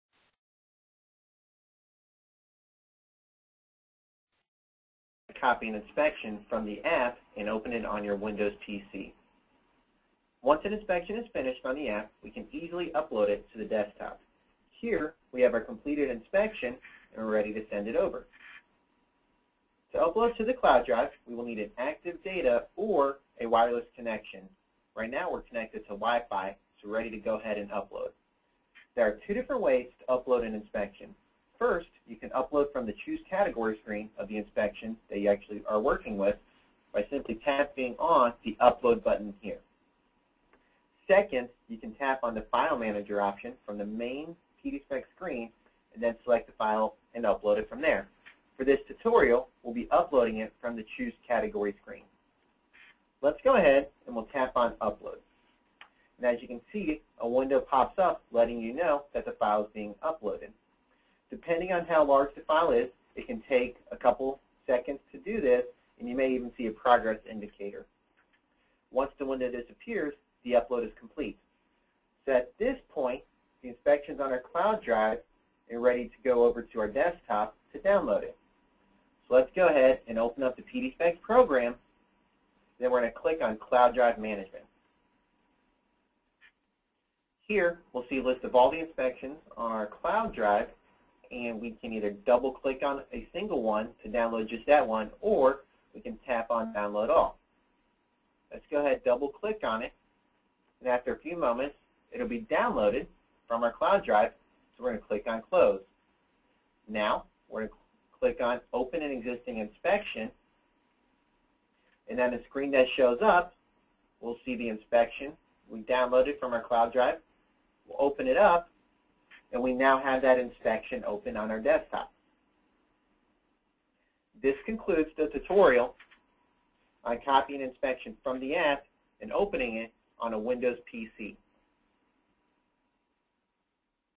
PDspect Commercial Inspection Software: Sending An Inspection From App To Windows PC
This tutorial will walk through the process of Sending an Inspection from app to windows PC with PDspect Inspection Software.